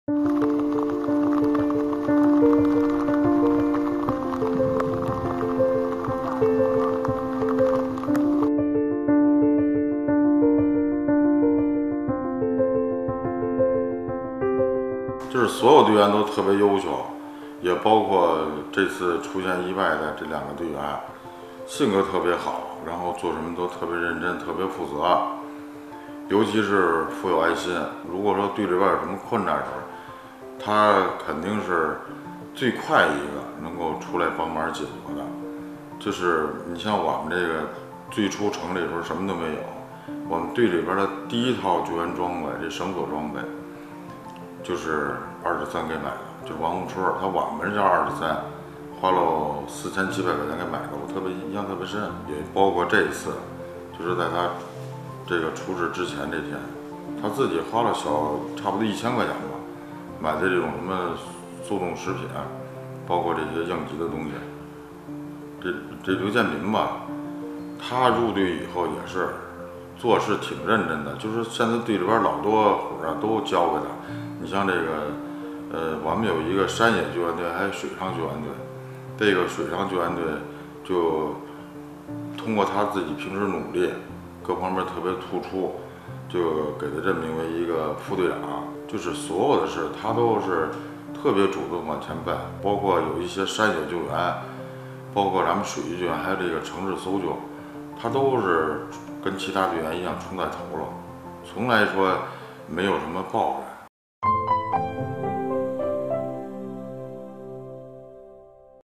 房山蓝天救援队队长回忆牺牲队友
7月30日夜间，一场暴雨侵袭了北京房山佛子庄，数十名村民被困。在这场艰难的营救行动中，房山蓝天救援队的王宏春、刘建民两名队员乘坐的皮划艇被急流掀翻，不幸牺牲。

近日，房山蓝天救援队队长陈海军在谈及两名队友时回忆称，王宏春做事特别认真特别负责，且富有爱心，救援队成立后的第一套救援装备就是王宏春花了4700元买的，而在此次救援前一天，她也准备了近一千元的食物和应急物资。而刘建民作为水上救援队的副队长，每次都主动冲在前面，从不抱怨。（李佳励 吕少威）